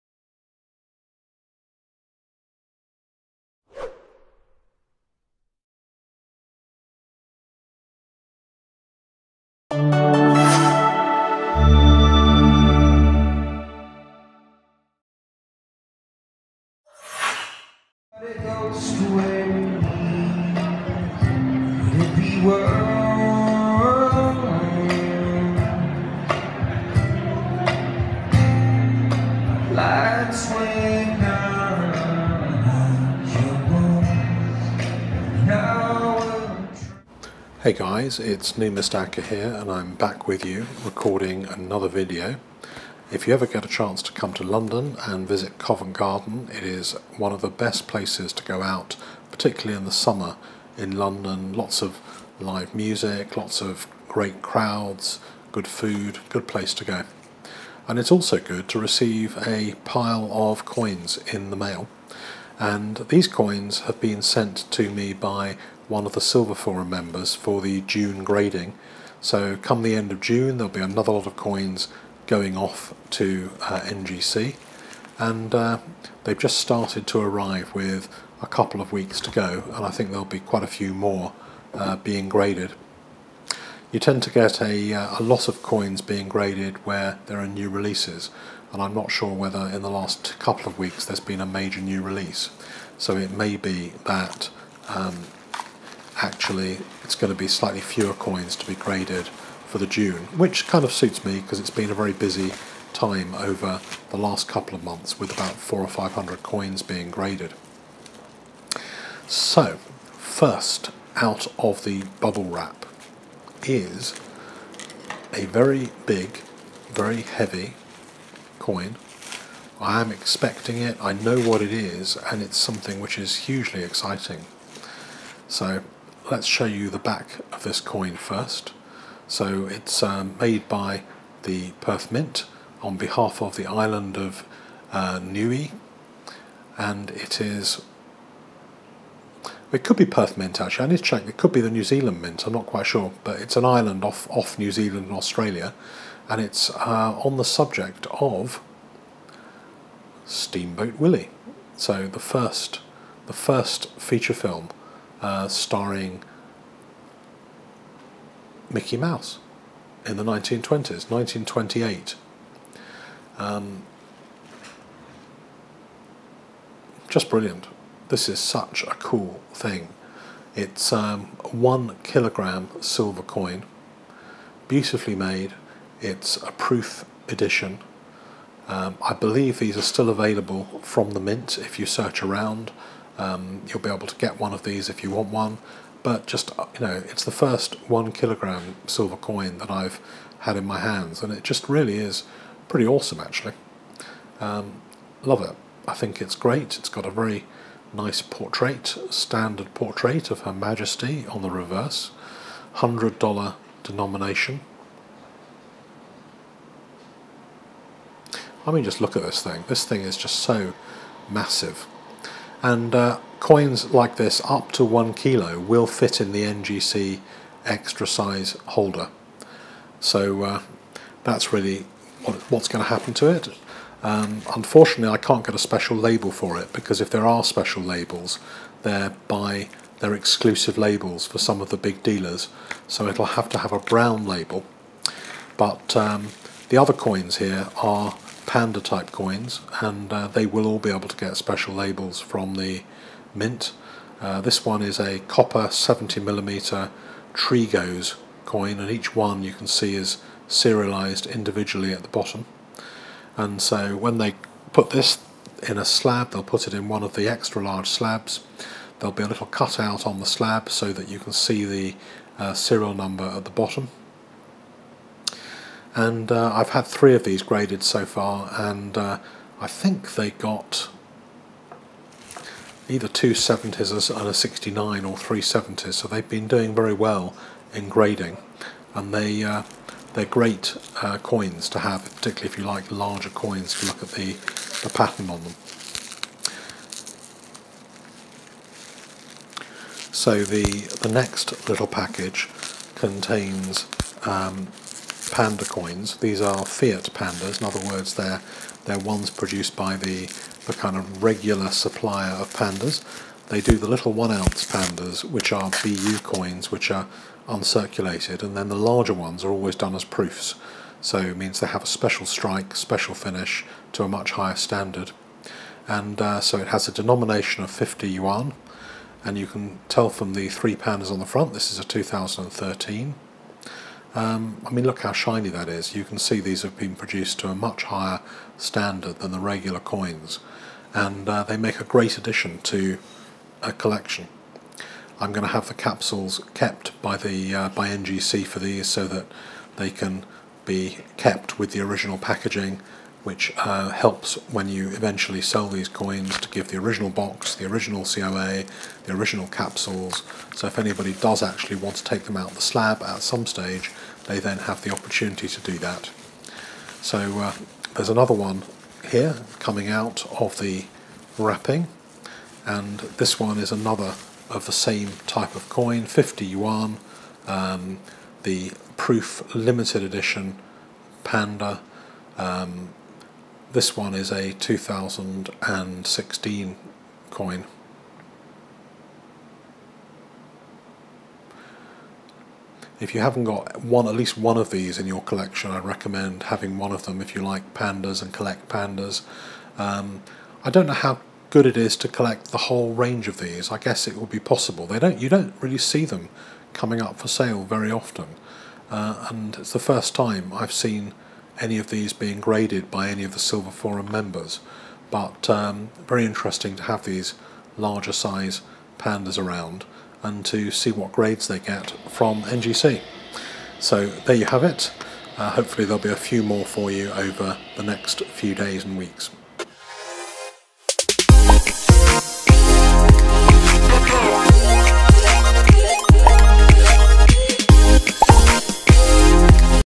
Mega Coins arrive for Mega NGC Grading | 1Kg Proof Steamboat Willie!!
Gian Pandas, Giant Mickey Mouse, Giant Copper. All the big game is live on the Numistacker Channel

334 Please support and visit channel sponsor The Silver Forum for the best news and views around.